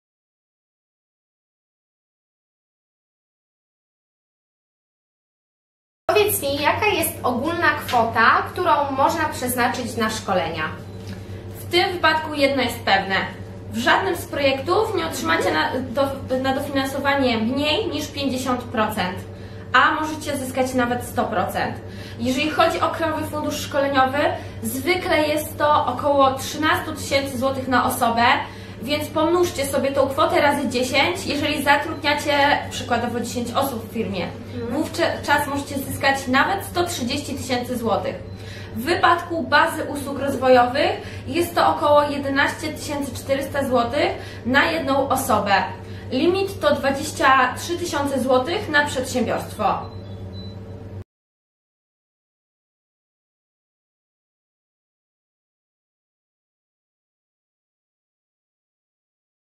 Jaką kwotę mogę otrzymać w ramach dofinansowania?
YT#17 W zależności od projektu możesz otrzymać nawet 50 000 zł dla siebie i swoich pracowników. Sprawdź szczegóły!